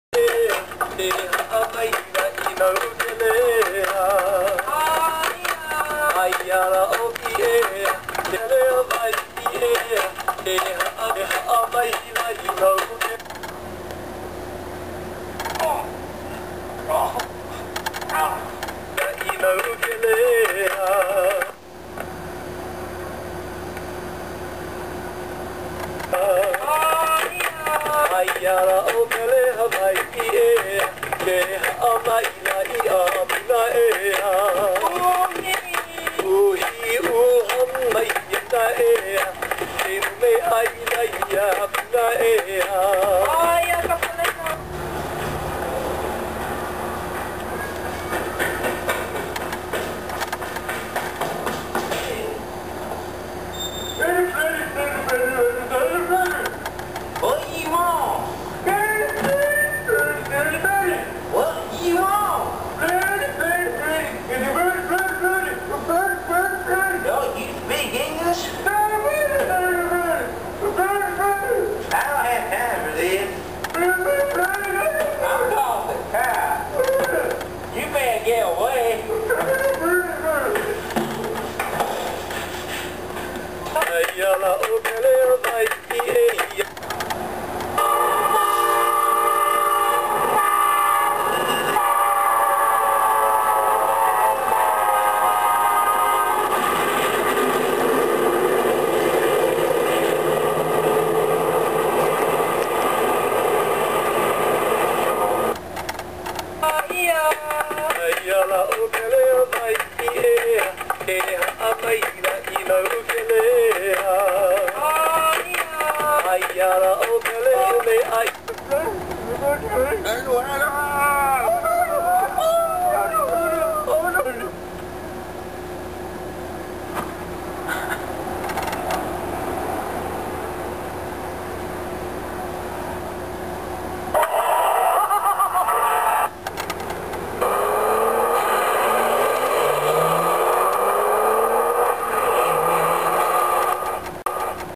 run ski boy run
an exchange student runs into trouble